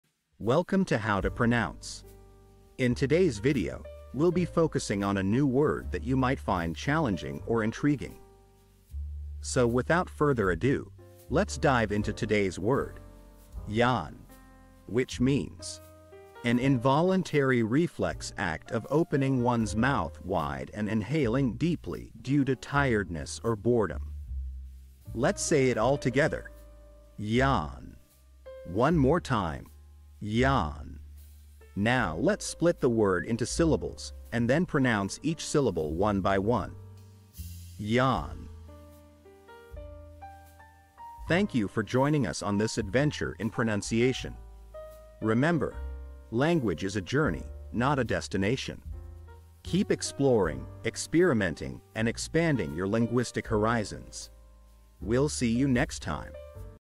How to pronounce Yawn
🌍 Learn a new language now Master the pronunciation of 'Yawn' Yawn 📖 - which means : An involuntary reflex act of opening one's mouth wide and inhaling deeply due to tiredness or boredom 📝 - with this quick and clear tutorial by How To Pronounce!

In this video 📽️, we'll guide you through the correct pronunciation of 'Yawn' in English, using native speaker examples 🗣️👂 and easy-to-follow phonetic breakdowns 🎵. Whether you're learning English for travel ✈️, business 💼, or just for fun 🎉, our guide will help you perfect your pronunciation in no time ⏰.